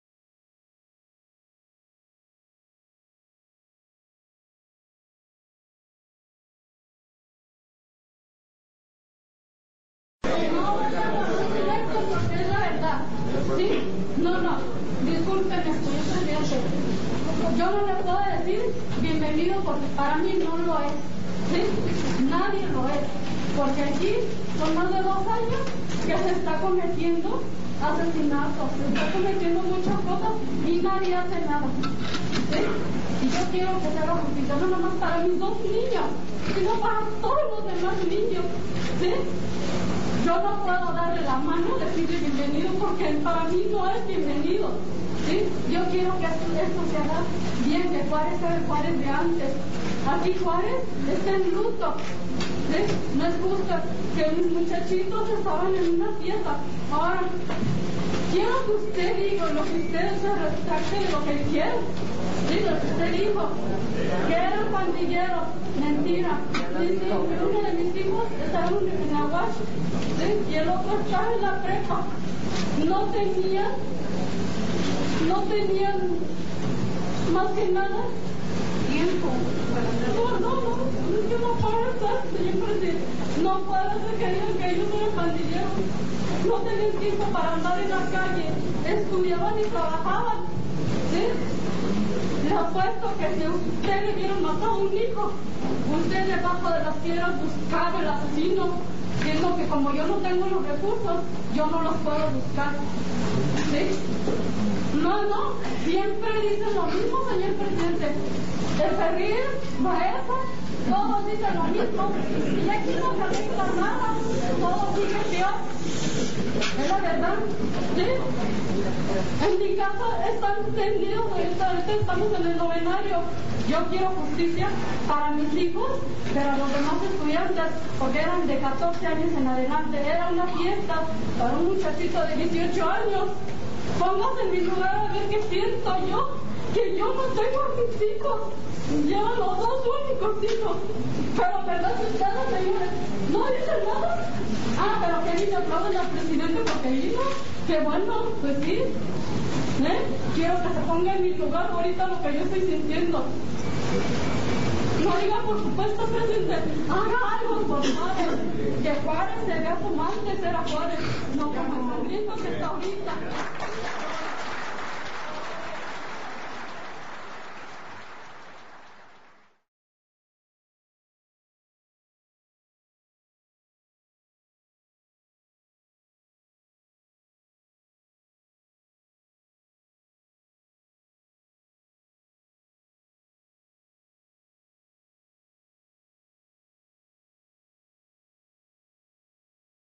INTERPOL Archive: 10282017-E15 Felipe de Jesús Calderón Hinojosa (1962-2011)
Felipe Calderon The Butcher of Morelia aka "The Cauldron" (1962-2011)

Felipe de Jesús Calderón Hinojosa (18 August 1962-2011)  was a Mason Mexican politician who served as President of Mexico from 1 December 2006, to 30 November 2011 after which he was brought to trial and convicted of murder in every degree, mass murder, serial murder.  He was an accomplice to genocide and ethnic  cleansing, child and human trafficking, and the transport and sales of drugs in excess of one billion dollars.

Jul 27, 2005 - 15, is just one of more than 120,000 children who have gone missing in Mexico, a country of 104 million people .